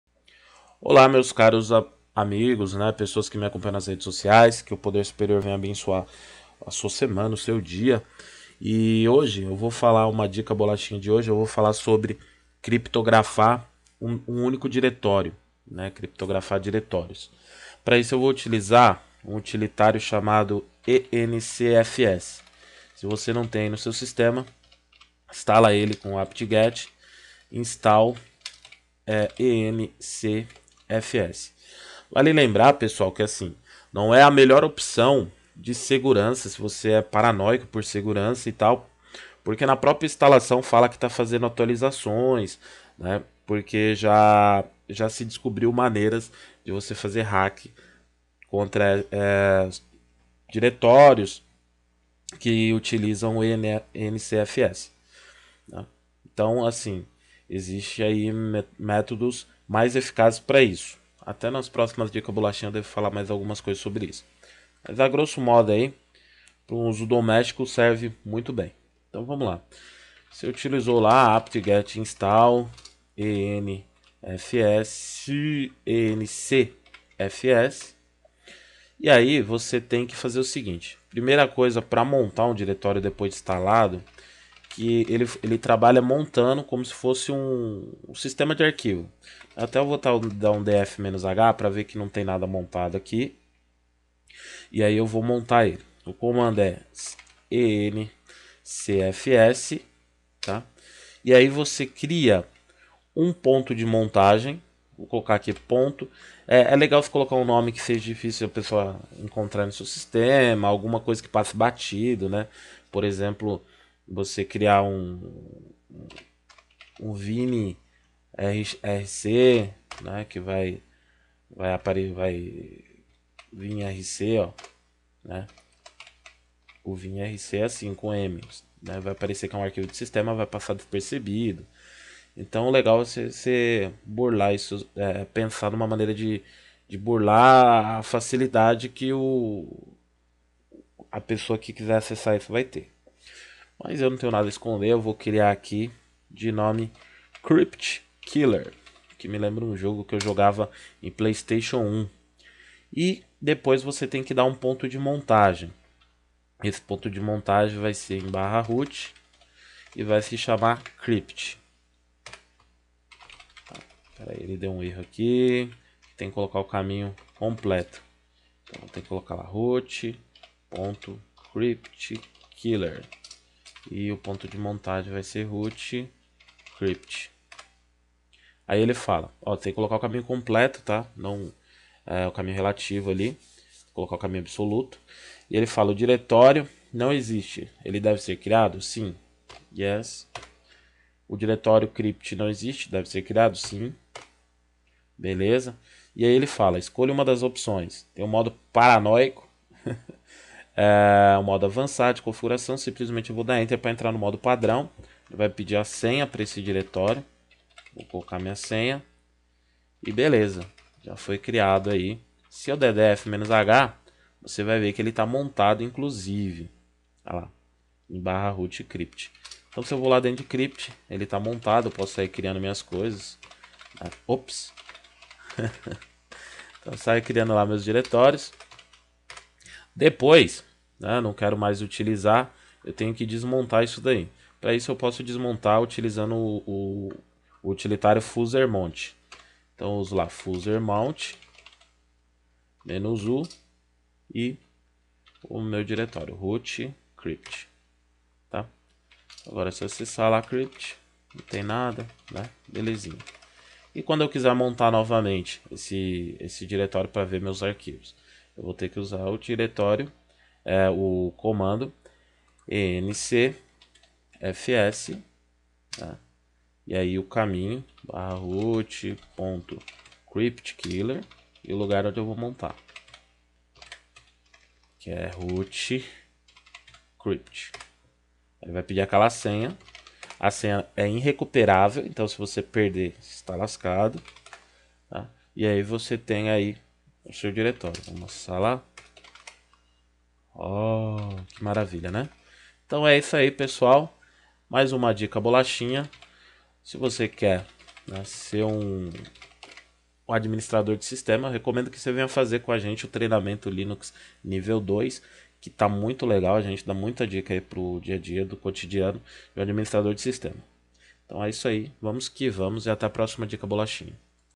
Dica bolachinha 31 - Criptografar diretórios no Linux.
Criptografar diretórios no linux, usando o encfs. (Mesmo que é usado na instalação do ubuntu).

Quer ser um administrador de sistemas linux?